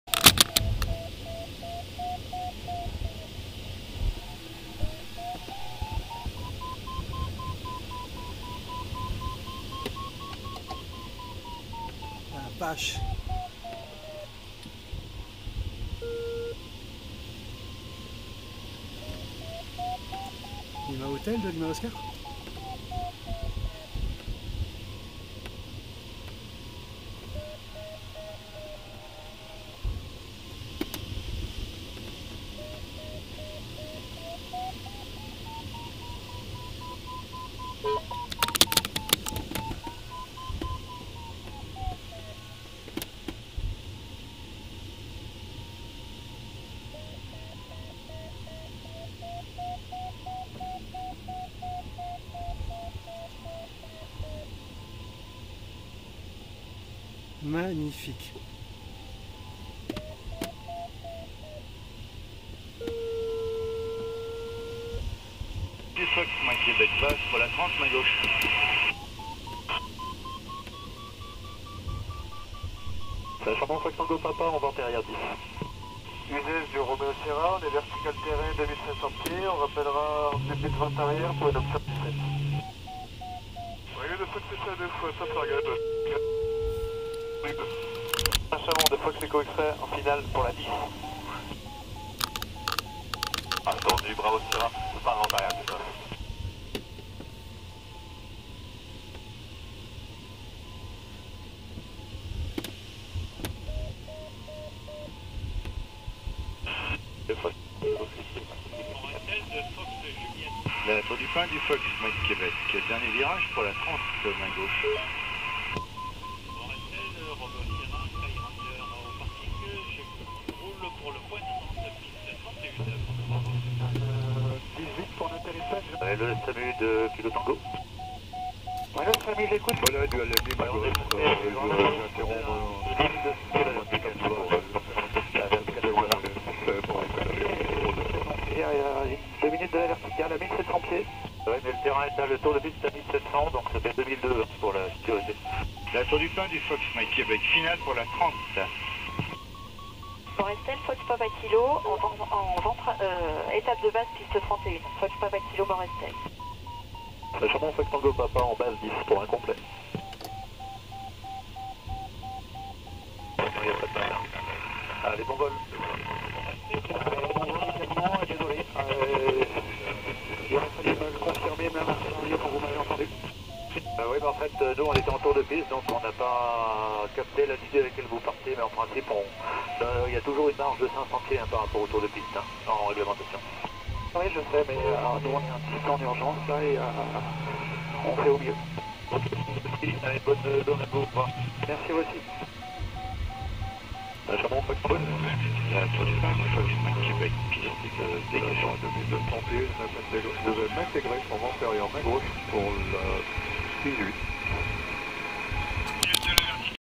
Vol en planeur - Spirale avec des cigognes -
Vol en planeur - Pegase C101A - le 4 septembre 2021
Partage d'une ascendance avec un groupe de cigognes en cours de migration vers le sud , aux alentours de cessieu (38) - LFKP